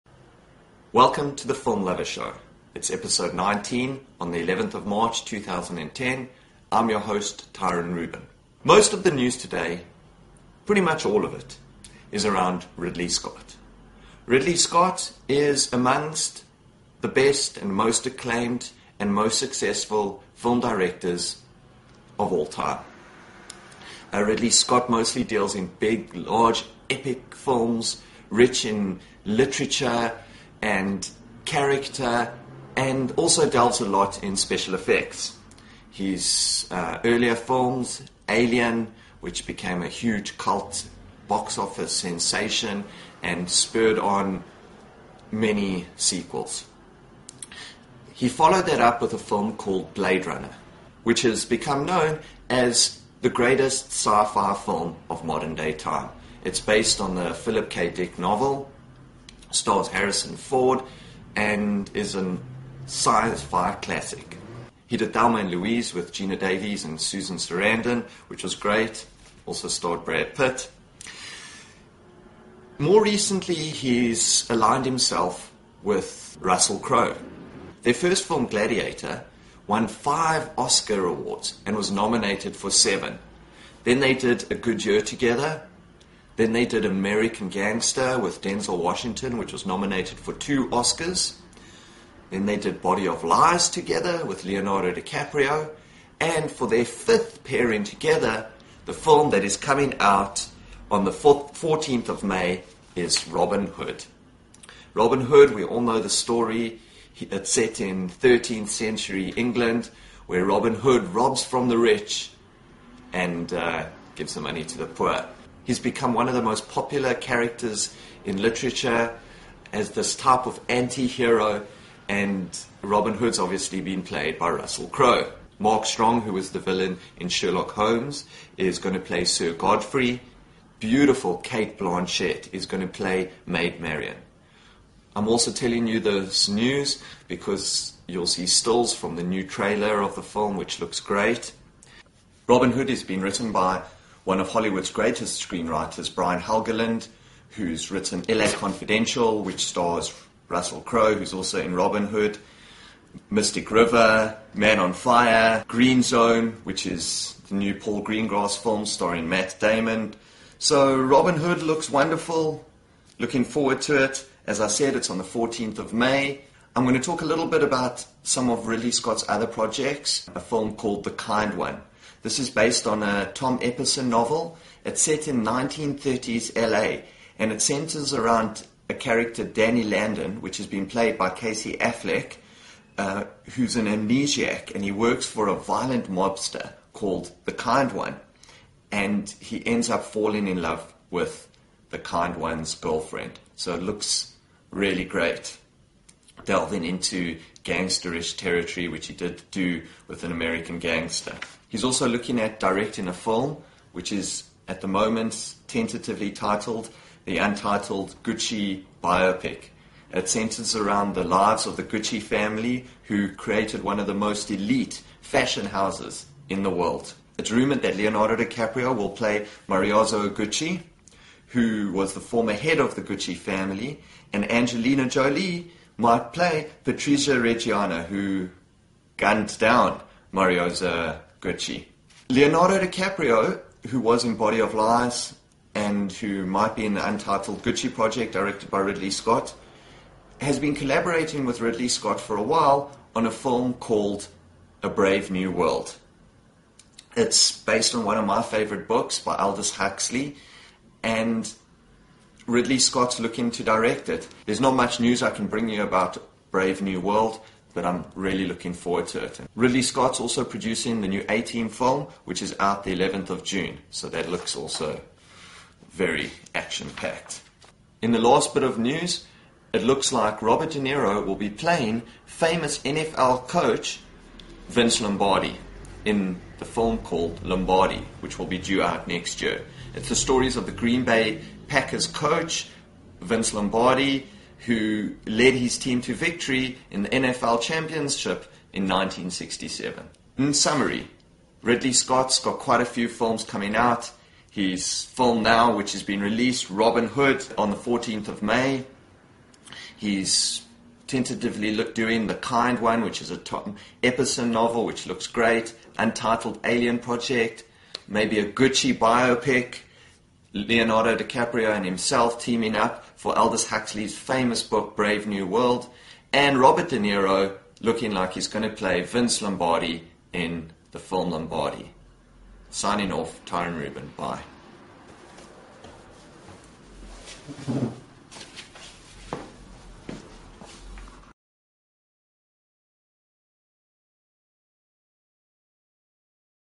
The Film Lover Show Episode 19 on 11 March 2010
- Ridley Scott's Robin Hood is released 14 May 2010 and the trailer it out. Ridley Scott plans Untitled Alien Prequel, The Kind One & Brave New World.
- De Niro to play NFL coach Lombardi